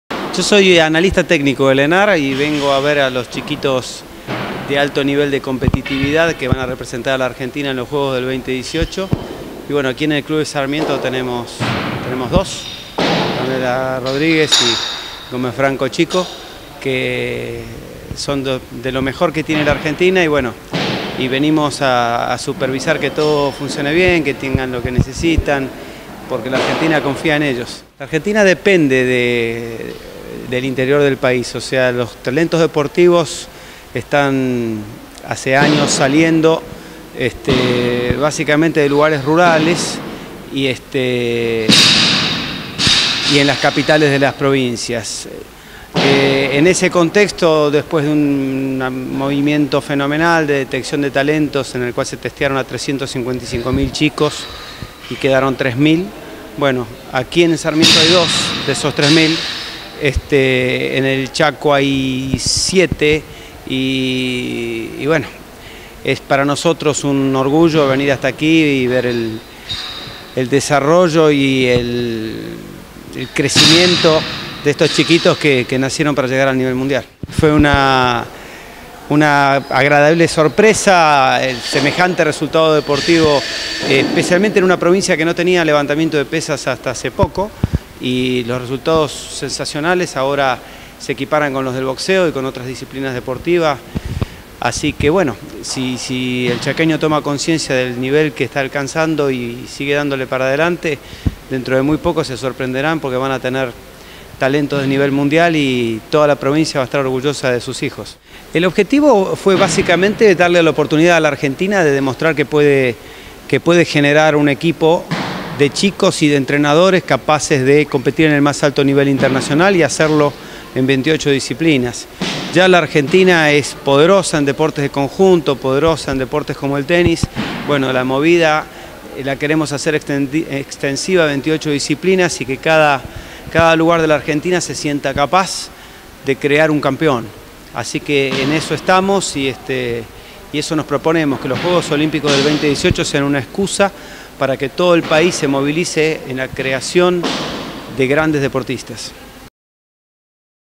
NOTA HORACIO ANSELMI ANALISTA TECNICO DEL ENARD
El Licenciado Horacio Anselmi visitó este lunes las instalaciones de la Escuela de Pesas del Club Sarmiento pensando en los Juego Olímpicos de la Juventud 2018 a llevarse a cabo en la ciudad autónoma de Buenos Aires.
La visita se debe a que la institución Aurirroja cuenta con dos jóvenes promesas en la disciplina de levantamiento Olímpico de pesas, Antonela Rodriguez y Gomez Franco.